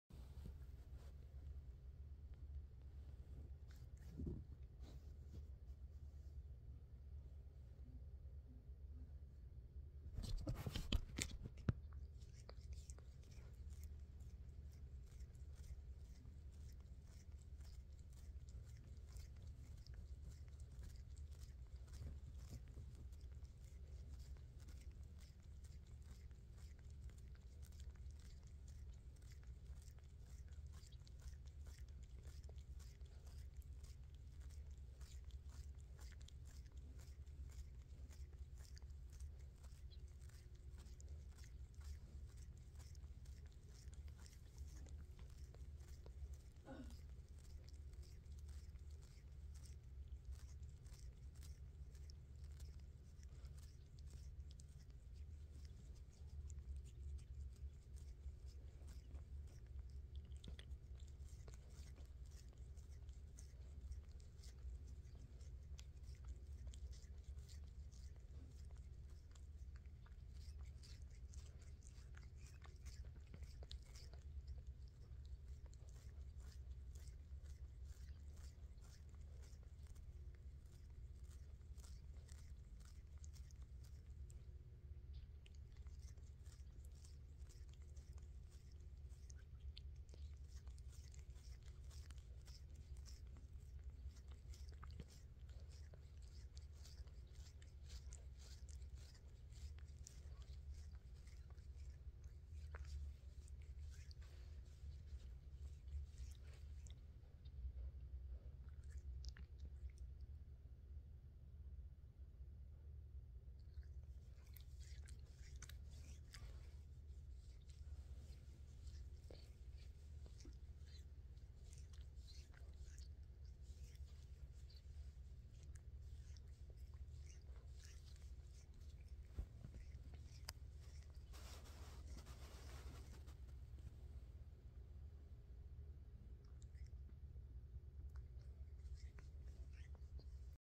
ASMR Cat Grooming.
Smooches is having an afternoon grooming session.